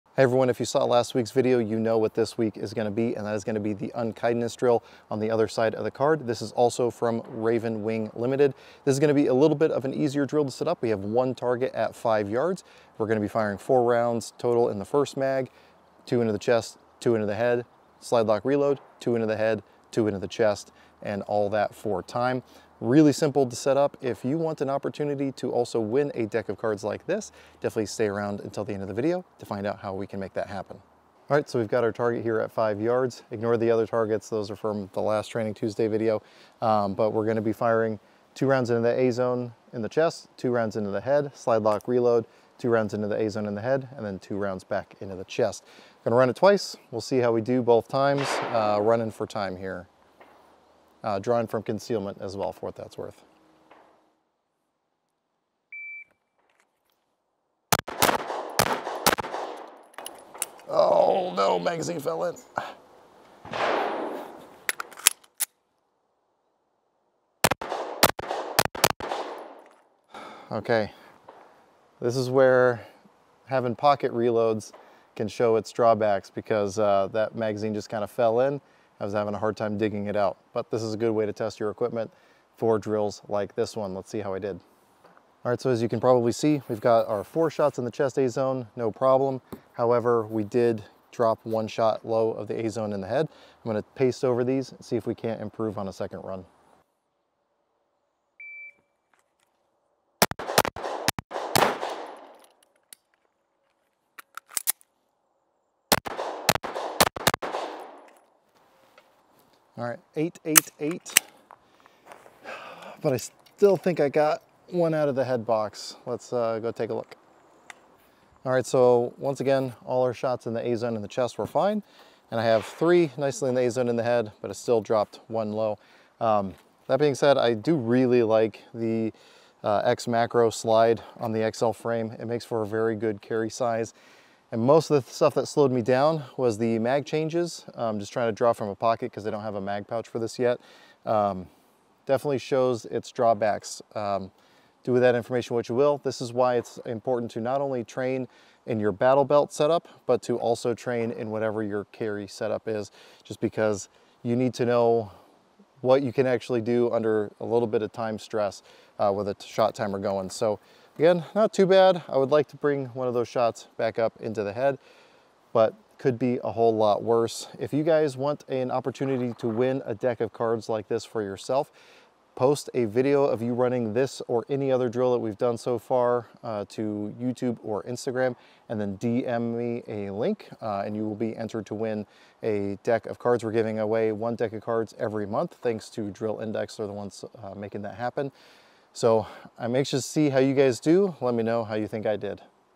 Unkindness Drill - Training Tuesday
In continuation of the other Raven Wing Limited drill from last week, we have the Unkindness drill. This is another simple and fun drill that tests precision and speed.

If you’re interested in picking up one of these decks for yourself, check out